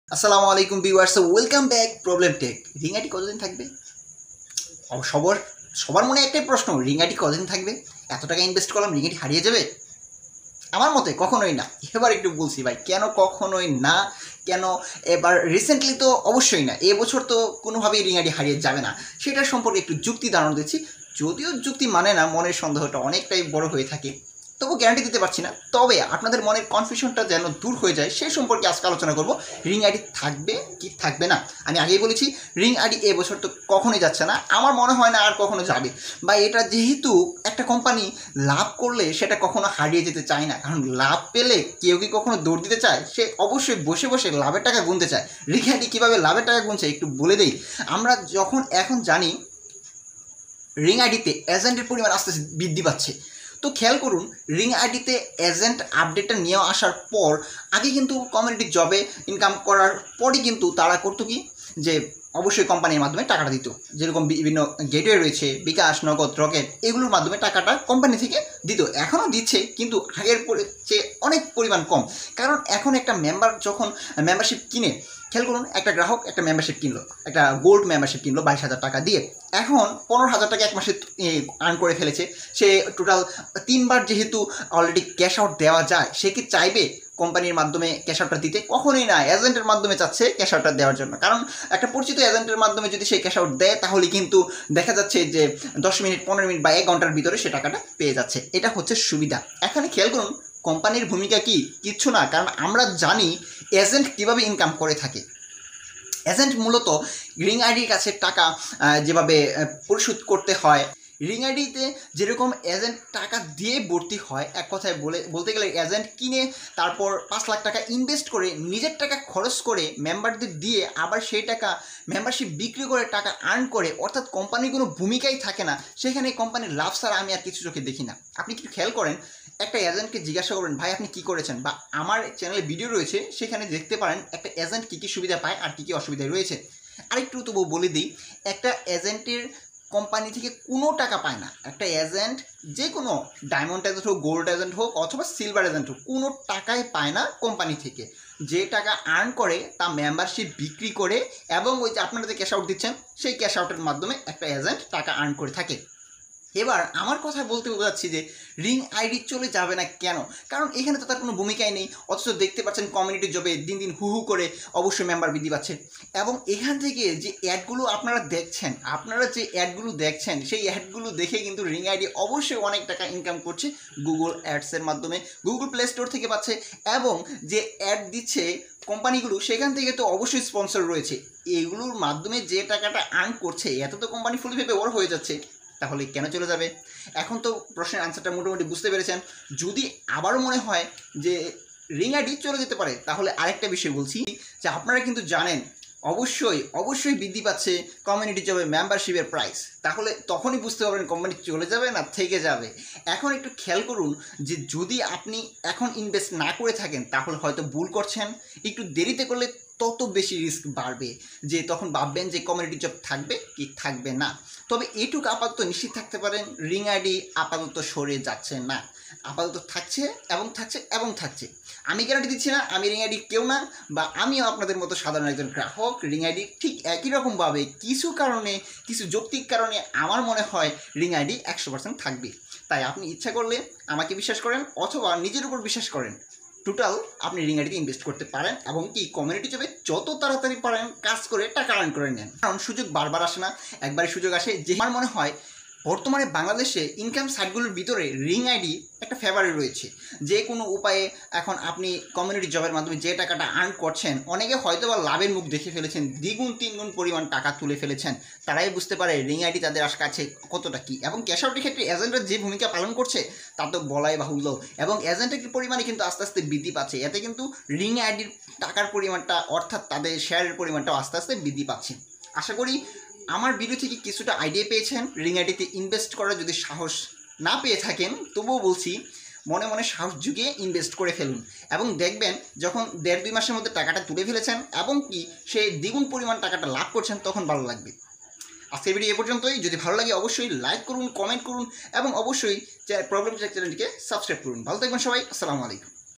Ring ID যে যে কারণগুলোর জন্য থাকবে। কেনো রিং আইডি হারিয়ে যাচ্ছে না।রিং আইডি কেনো বন্ধ হবে না।
বিস্তারিত ভিডিওতে রয়েছে। তাই জেনে নিতে পারবেন।